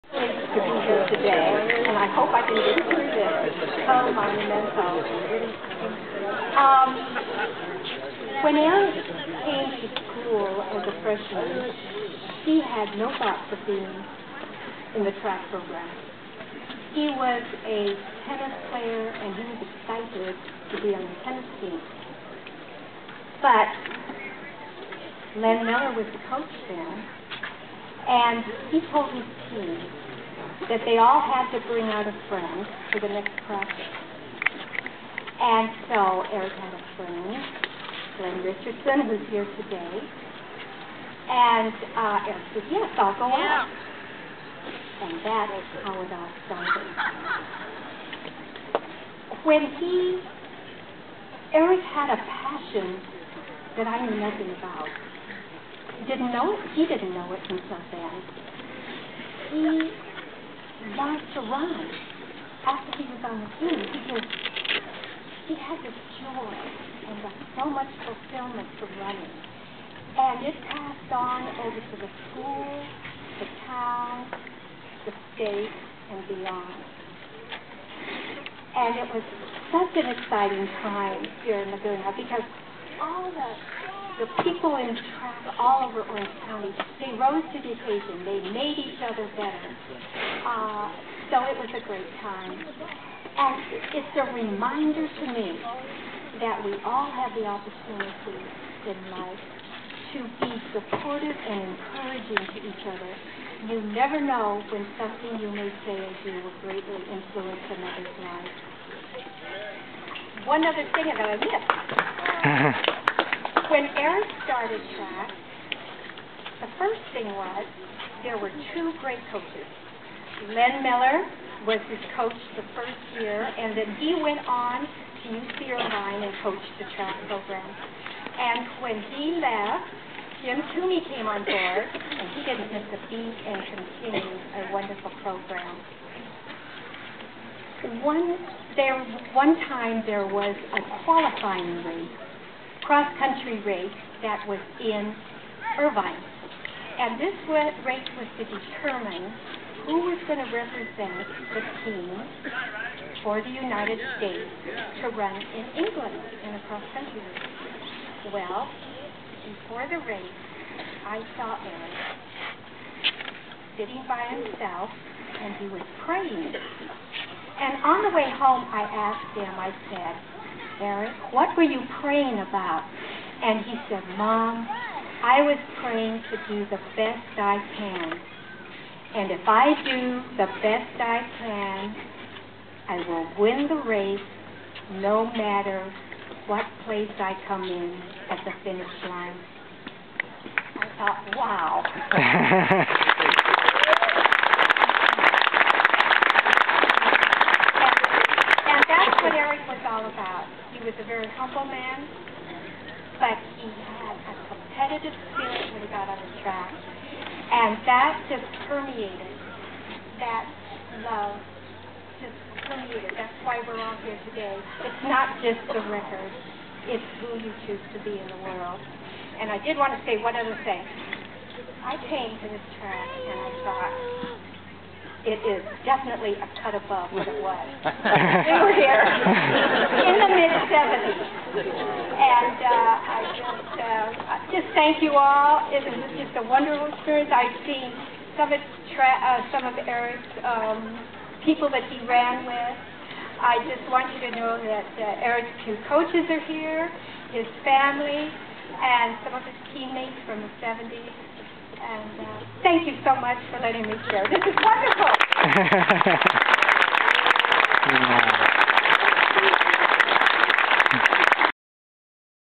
Eric Hulst Track - Sharon Sandin Dedication Speech March 20,2010
March 20, 2010  My step-mom Sharon making a brief speech dedicating the new Laguna Beach High School Eric Hulst Track in honor of her son Eric who was a HUGE nationally known track star back in the 70's.